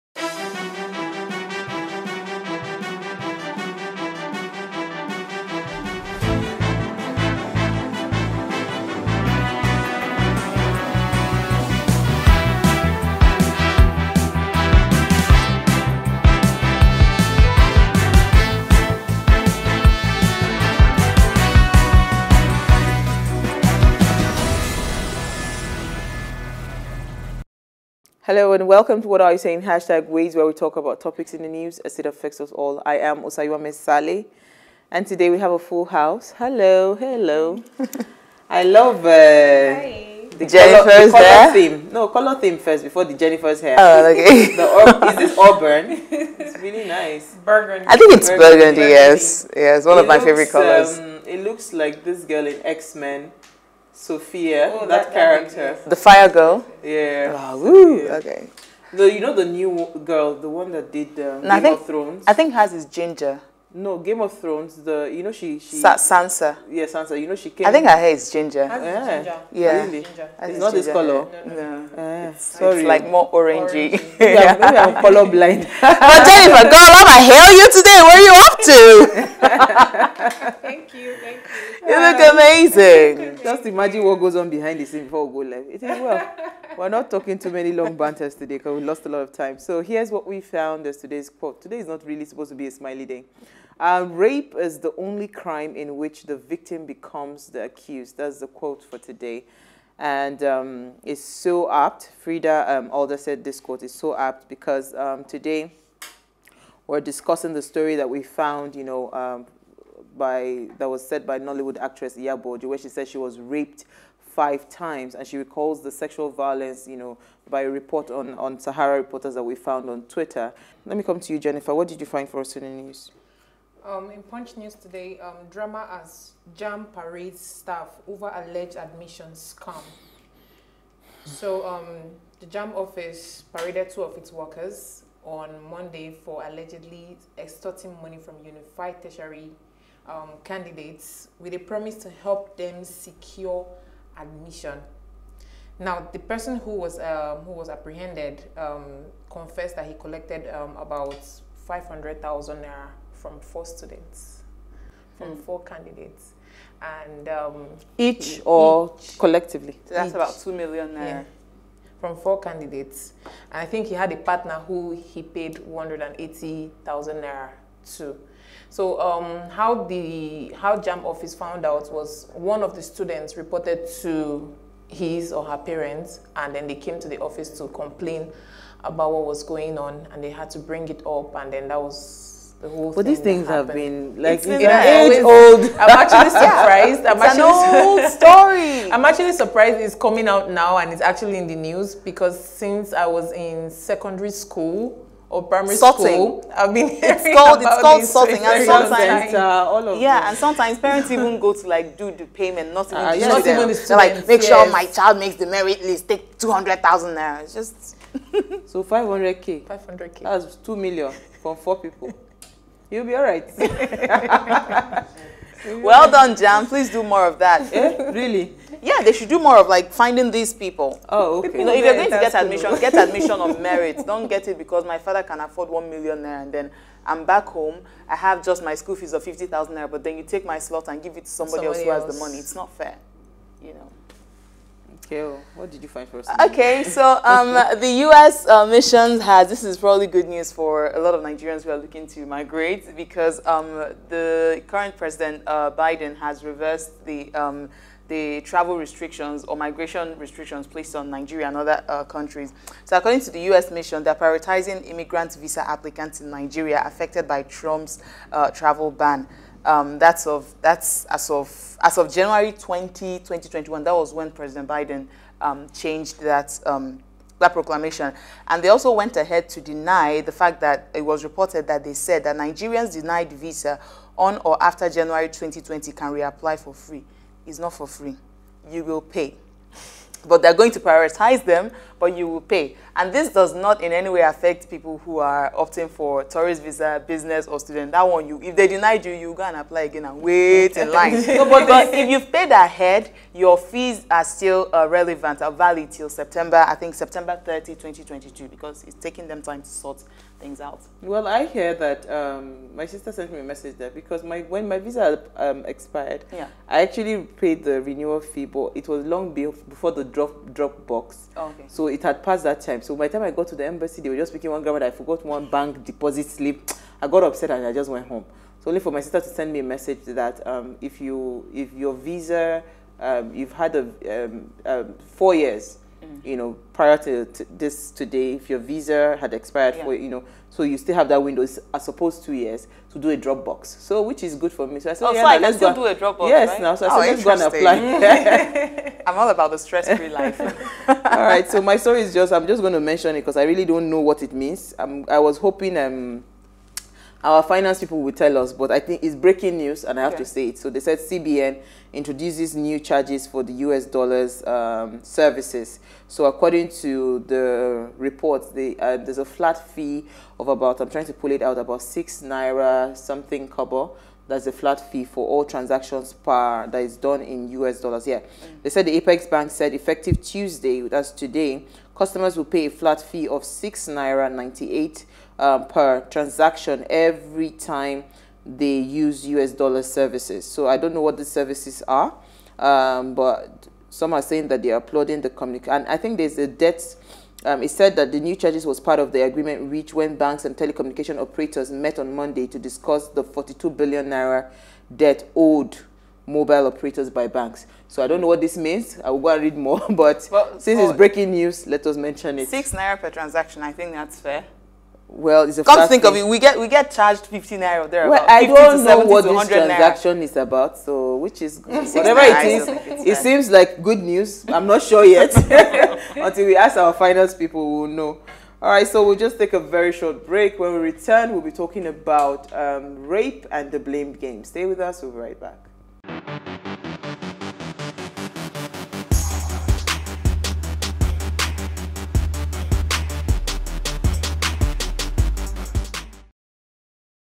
What's In The News - CBN Introduces New U.S Dollar Service Charges
Here’s what we found as today’s quote: Rape is the only crime in which the Victim becomes the Accused - Freda Adler

What did you find for us in the news?